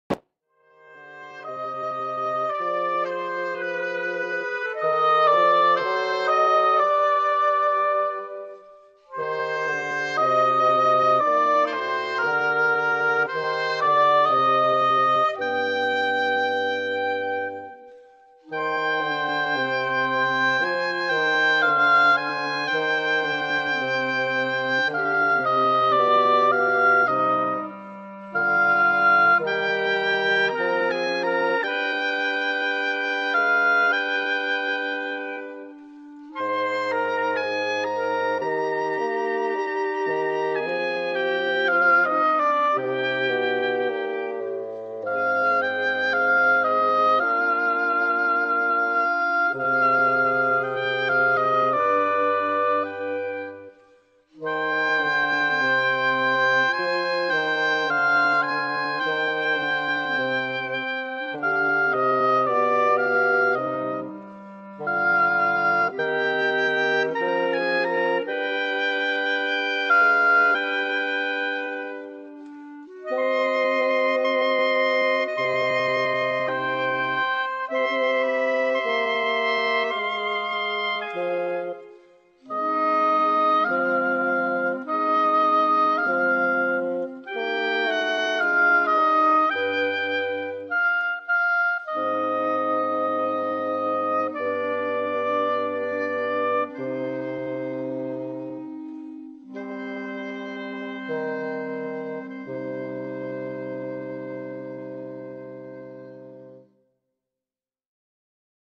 Besapiés del Cristo de la Coronación de Espinas (El Valle) 2015
Besapiés del Cristo de la Coronación de Espinas, de la Hdad del Valle, en la Iglesia de la Anunciación, los días 14 y 15 de Noviembre del 2015